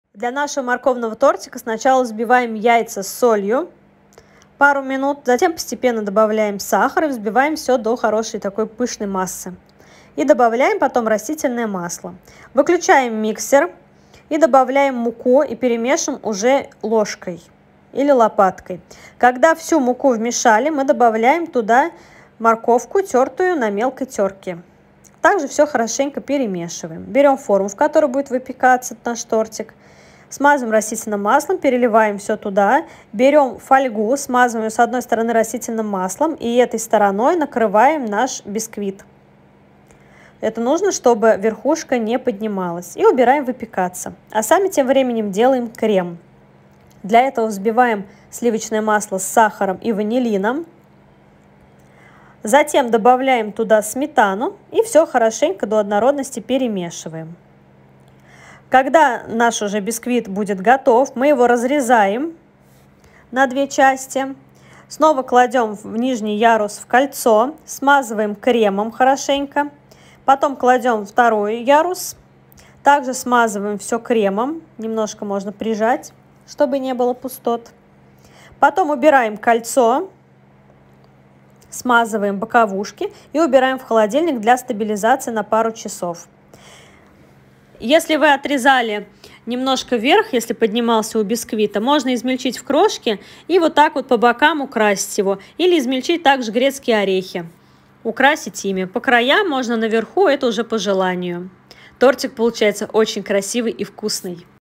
Морковный торт
Ингредиенты:
Для коржей:

Для коржей взбиваем яйца с солью, затем постепенно добавляем сахар и взбиваем до пышности. Добавляем масло растительное. Затем добавляем туда муку с разрыхлителем и перемешиваем уже лопаткой. И туда же кладём тертую на мелкой тёрке морковь.

Выливаем в форму. Далее берём фольгу и одну сторону смазываем растительным маслом и накрываем ей наш бисквит. И убираем в духовку на 35-40 минут при температуре 180С.

Для крема взбиваем масло сливочное с сахаром и ванилином, затем добавляем сметану и все вместе взбиваем ещё пару минут.

Достаем бисквит, убираем фольгу даём остыть. После этого разрезаем пополам и нижний корж снова кладём в форму. Смазываем кремом и накрываем вторым коржом.

Убираем кольцо, смазываем кремом сверху и по бокам и украшаем крошкой от коржей и измельчёнными грецкими орехами.

Убираем в холодильник на час-два для стабилизации.

Приятного аппетита 😊